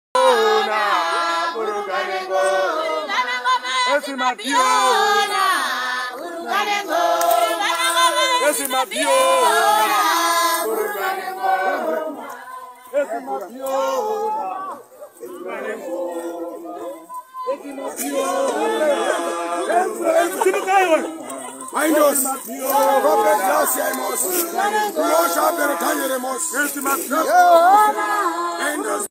This woman is possessed by the spirit of a hoe - See what she does to this man at church👀
They prayer for this woman and sprinkled her with holy water, what she did afterwards will shock you…